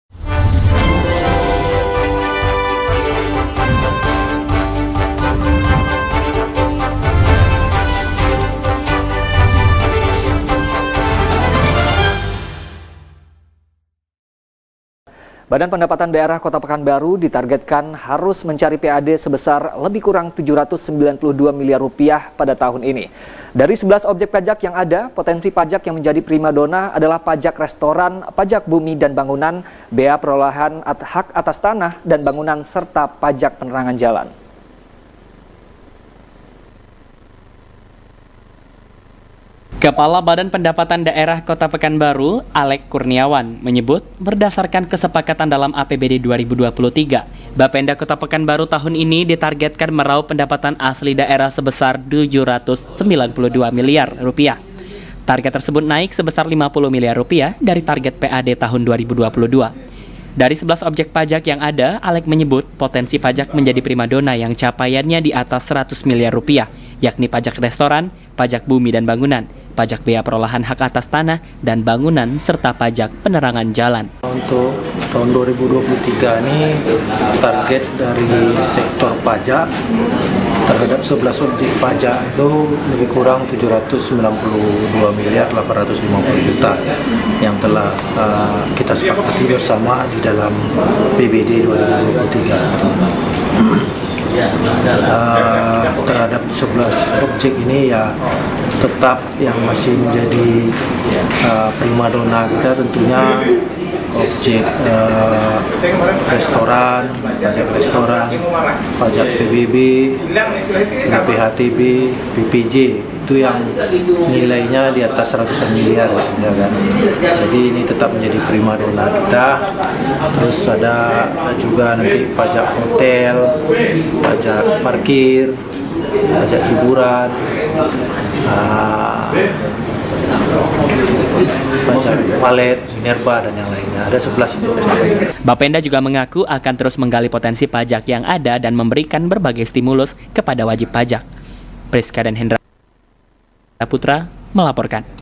TARGET PAJAK BAPENDA TAHUN 2023 DITETAPKAN SEBESAR 792 MILYAR RUPIAH
Badan pendapatan daerah kota pekanbaru, ditarget kan harus mencapai pad sebesar lebih kurang 792 miliar rupiah, pada tahun ini. dari 11 objek pajak yang ada, potensi pajak yang menjadi primadona adalah pajak restoran, pajak bumi dan bangunan, bea perolehan hak atas tanah, dan bangunan serta pajak penerangan jalan.